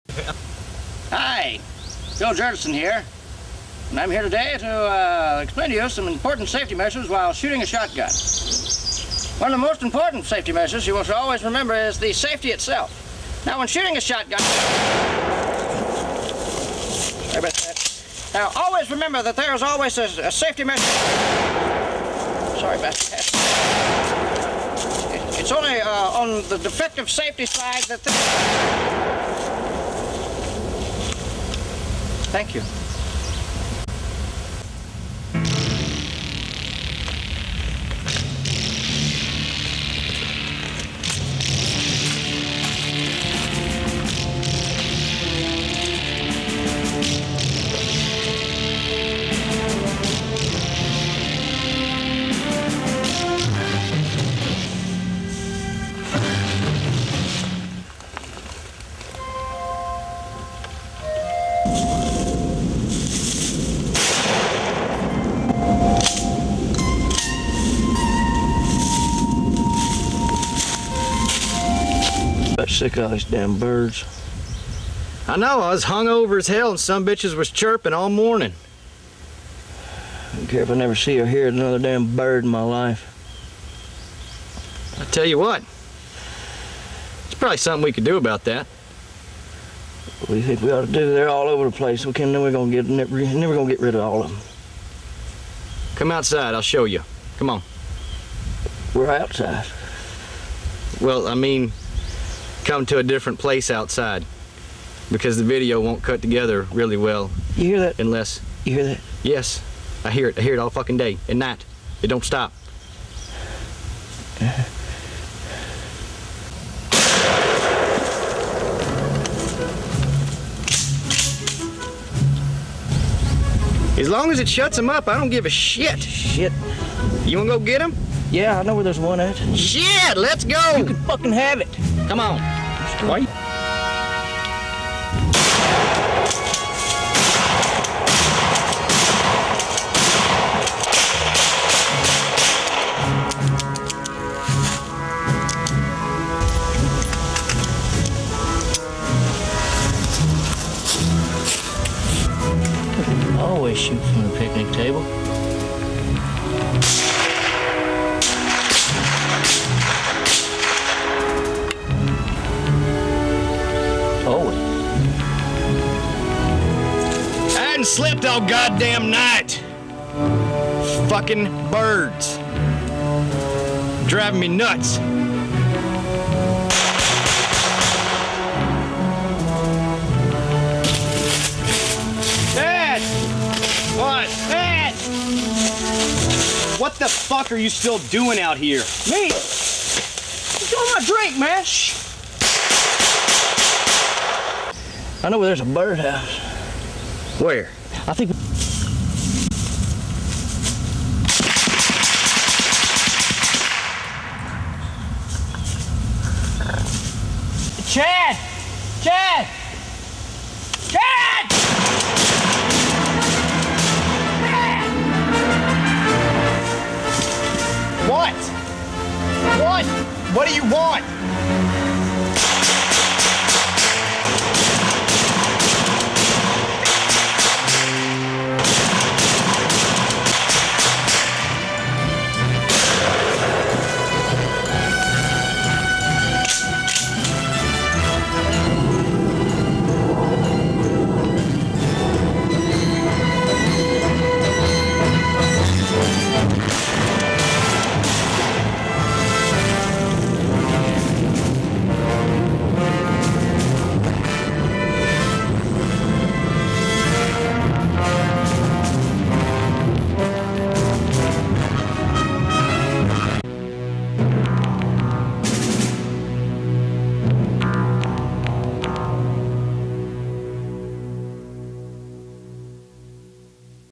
BIRDNEST HANGOVER - PICKLE TV
Killing birds in the nest with guns due to lack of sleep because of all the damn chirping.